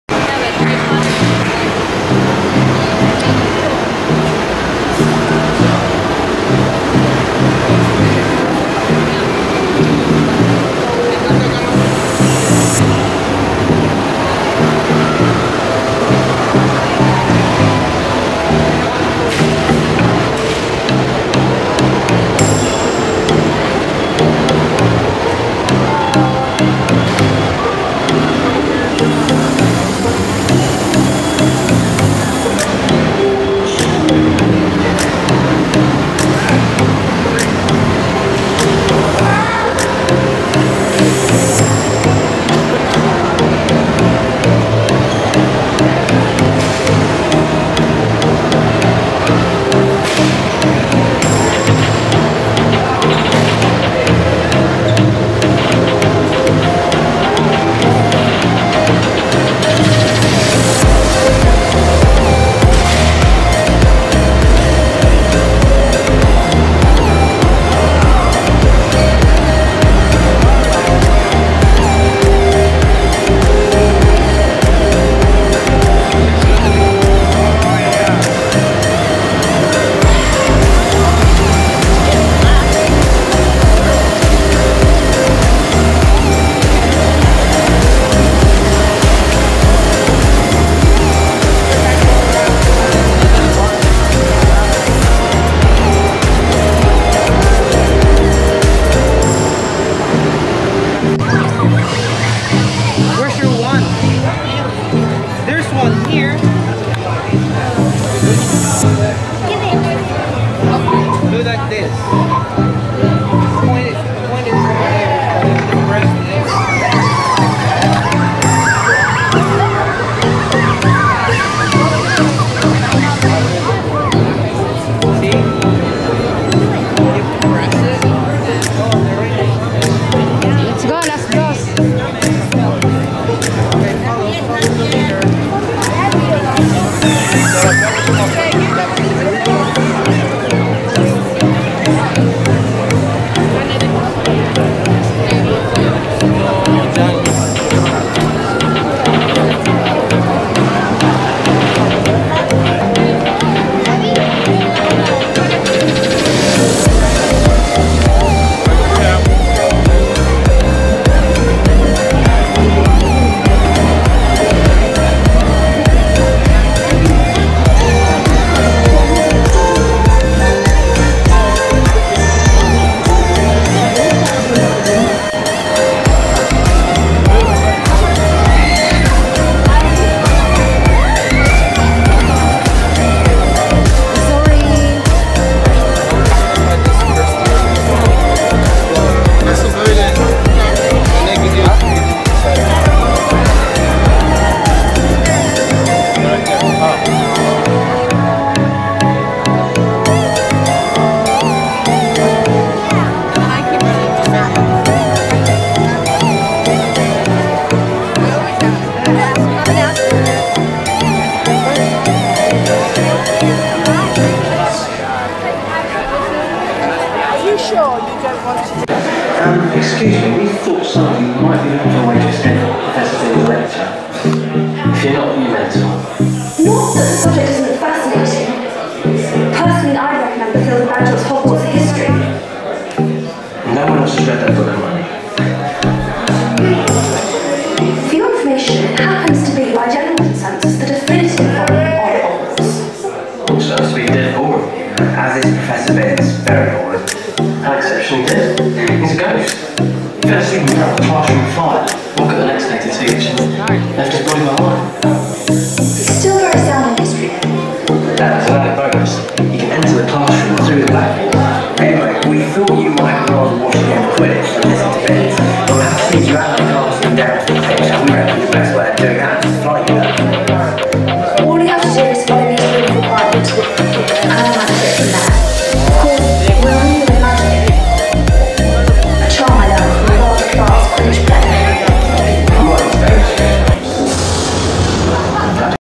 Riding Harry Potter Train @ Universal Studio
Hello everyone! Went to Universal Studio in Orlando, Florida with my family. The kids enjoyed it. Thank you so much.❤️